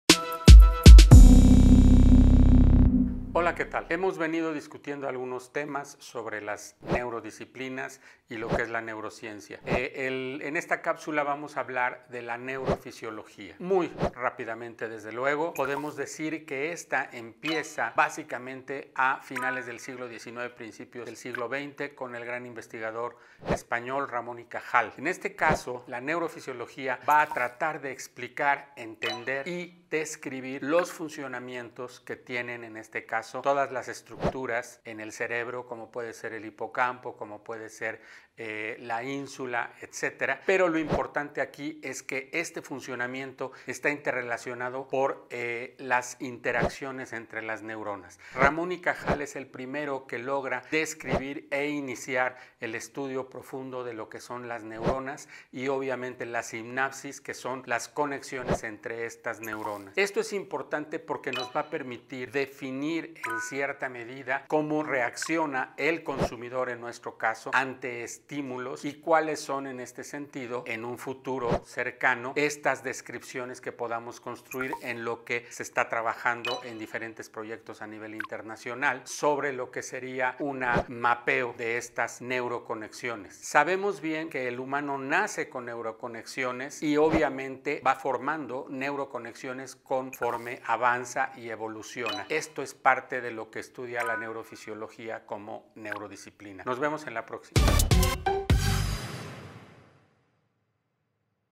La neurofisiología - 88 Segundos - Martín Diez
¿Qué tiene que ver la Neurofisiología con el marketing?, ¿por qué es importante para el consumidor?
Martín ​Díez nos explica en 88segundos

¡Síguenos la pista!